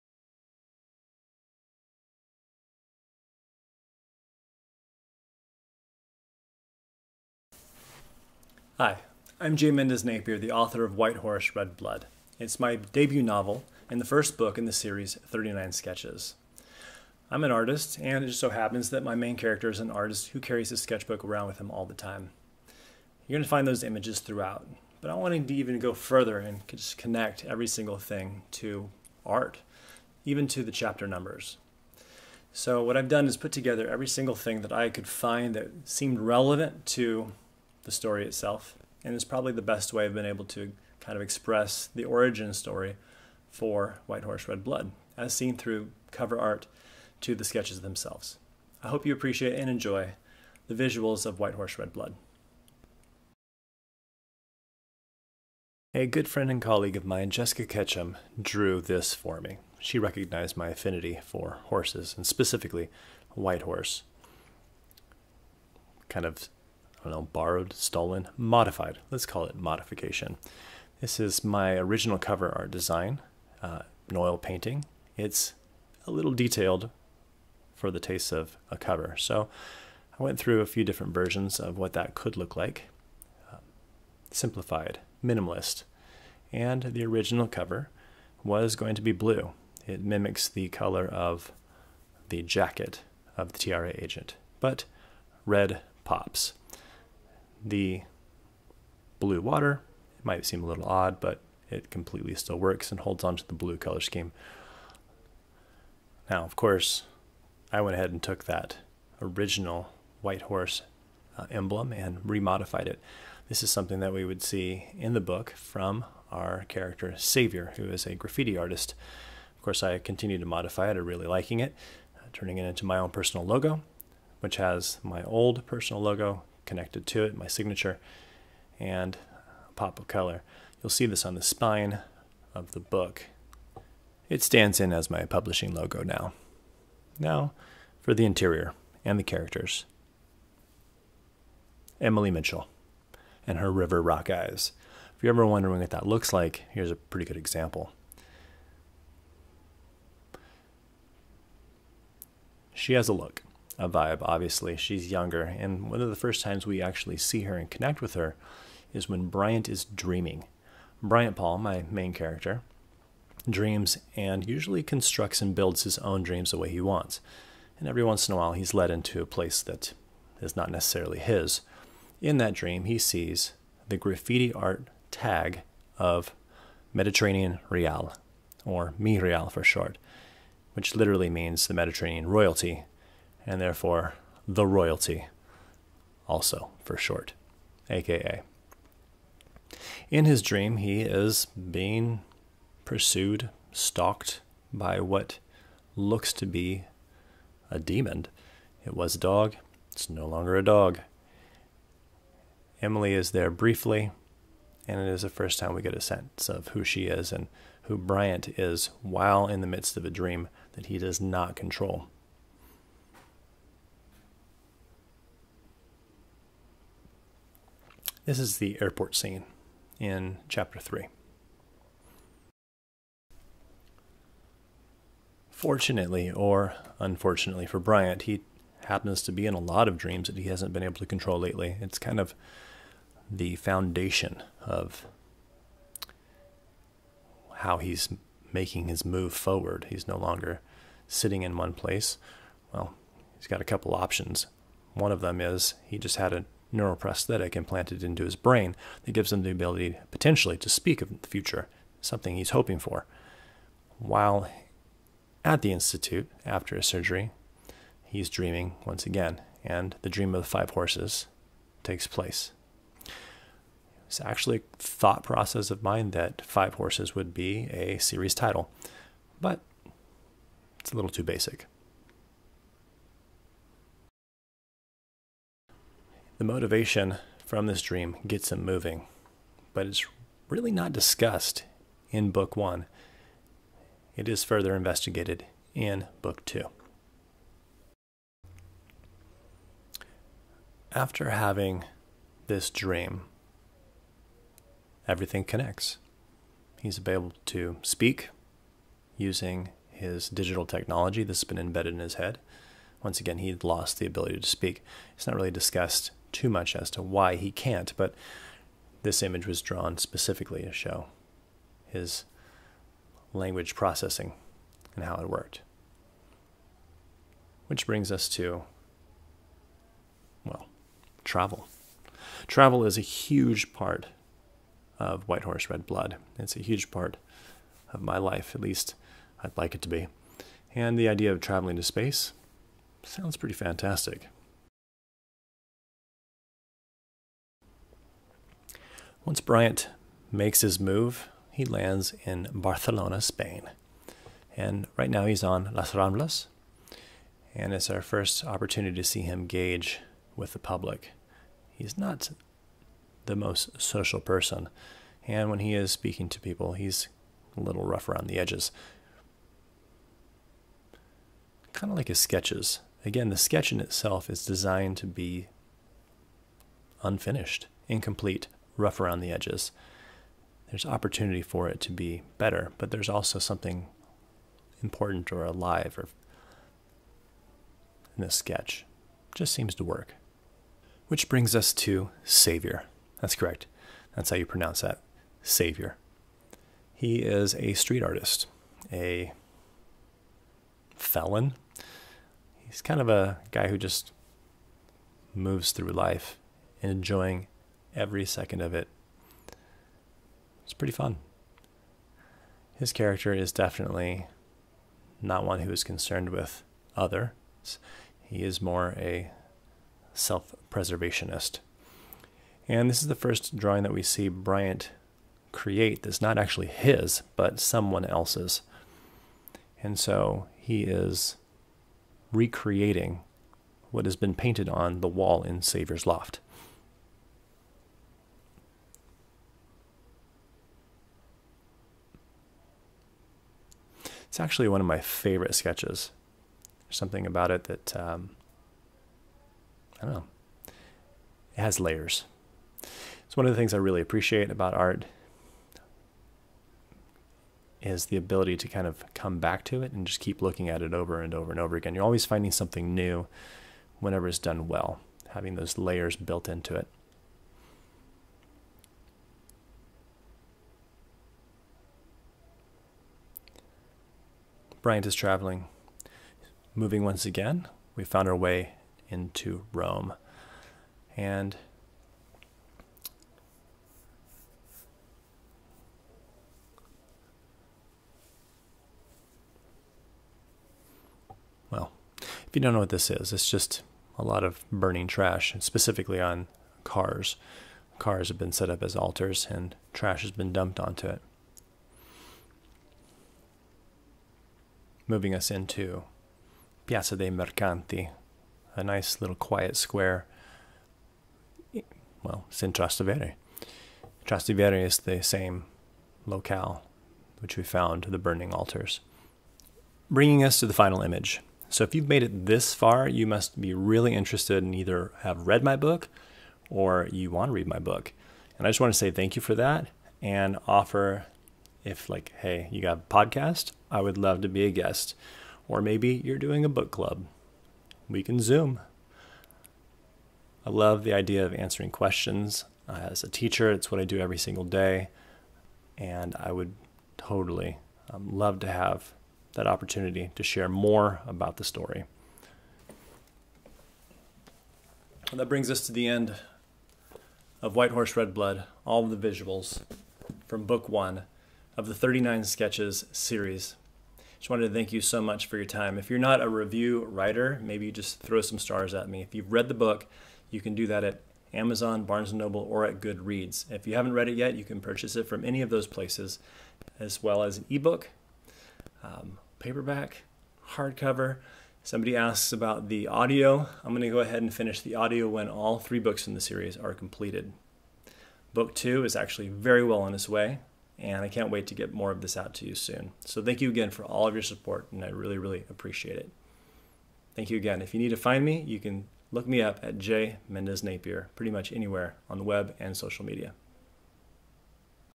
The Visuals of WHITE HORSE RED BLOOD - a novel by J. Mendez Napier
The near future sci-fi novel, WHITE HORSE RED BLOOD is out. Here, the author takes you through the premise of the story using the visuals from the book. From Cover Design and Artist Sketches - to Story Outline. (no spoilers)
Available through online retailers - Amazon and BN.com

Back of the Book description:
Neural-Prosthetics are the future of crossing lines. A future that now rests in the brain of Bryant Paul. A frustrated and distracted man, Bryant's three-year inability to speak has driven him to experimental surgery to restore vocalization. Today, he will wake up to an instrument connecting as promised - and so much more. He's received hope.

Fueled by this hope and his selfish behavior - Bryant will capitalize on his new advantage and set out as a fugitive to reunite with his wife and daughter. Not even the threat of death or his collusion with a government disruptor/terrorist will stop him.

Only two men stand to derail him now. A government agent tasked to hunt down enemies of the state, and the destructive, control obsessed Bryant himself.